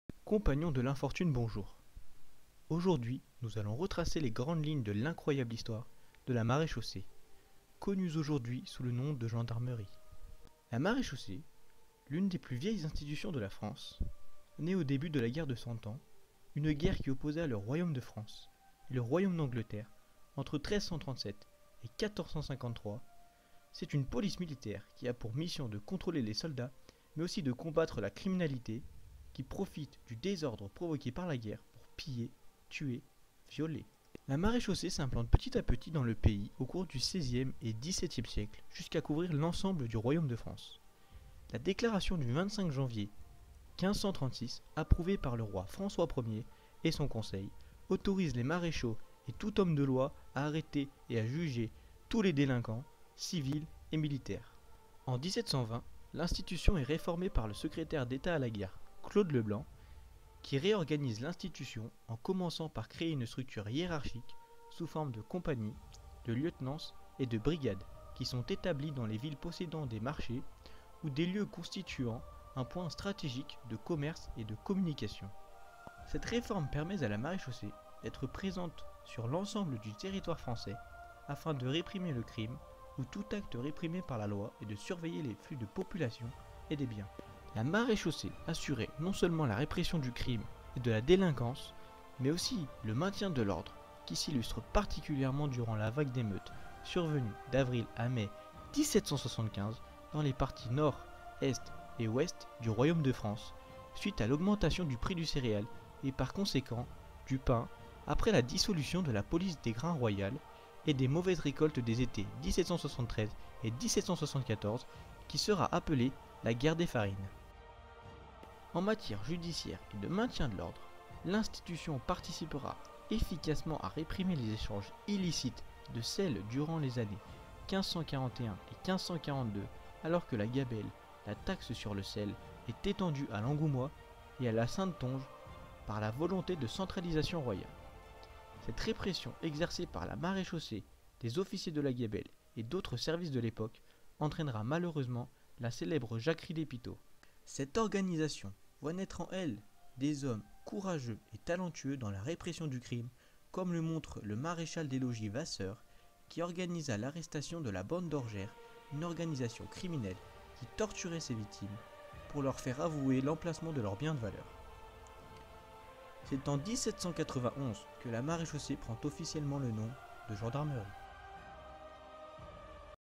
les grandes lignes de l'histoire : la maréchaussée
Dans cette courte vidéo, Daniel va nous retracer les très grandes lignes de la maréchaussée. De la guerre de 100 ans jusqu'à devenir la gendarmerie, Daniel nous racontera les grands événements de cette institution.